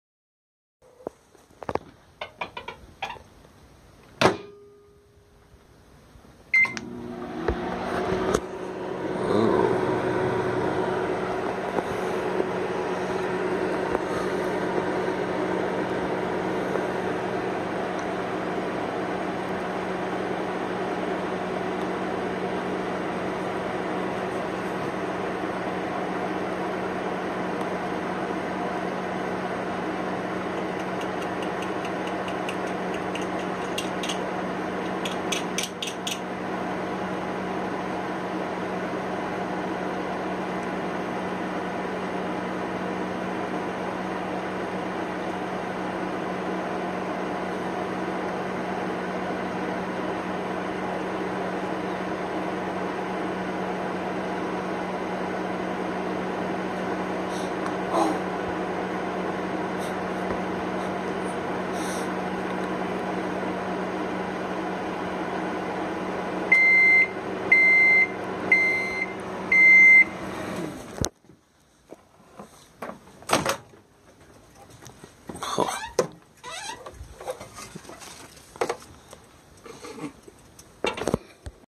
M&M in the Microwave
What happens to a brown chocolate M&M in the microwave after 1 minute.